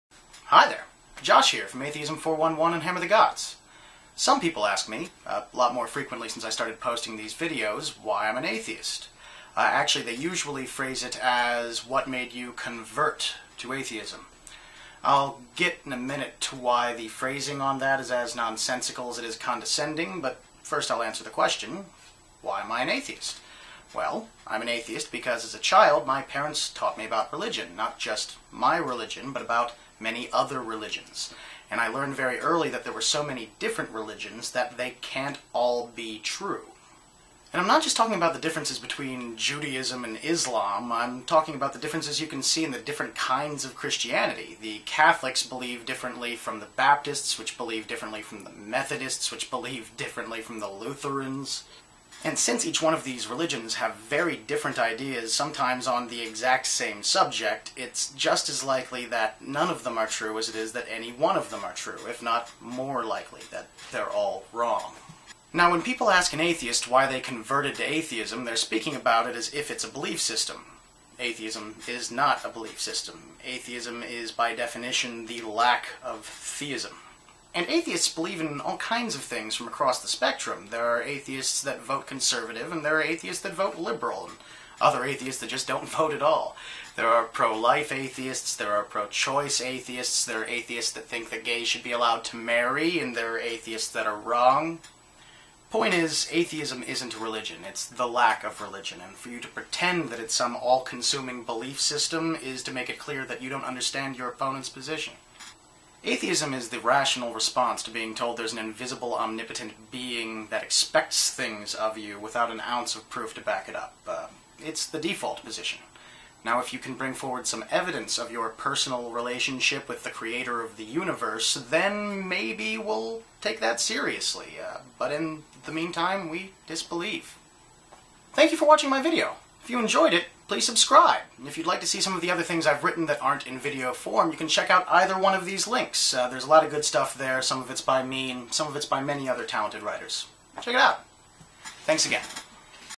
The Default Position - "Converting" to Atheism?
Josh Johnson explains why one can't "convert" to atheism, and why he disbelieves in all gods.

and